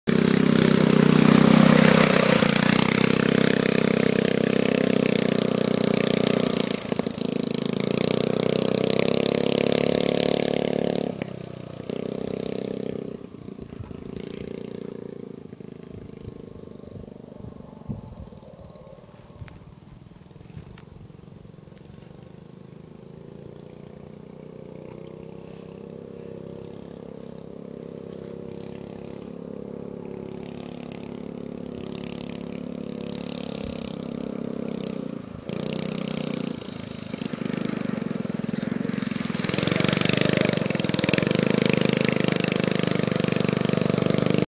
Friend Driving my Go-Kart Around!!!
Out on a beautiful day driving my go-kart around like crazy!!!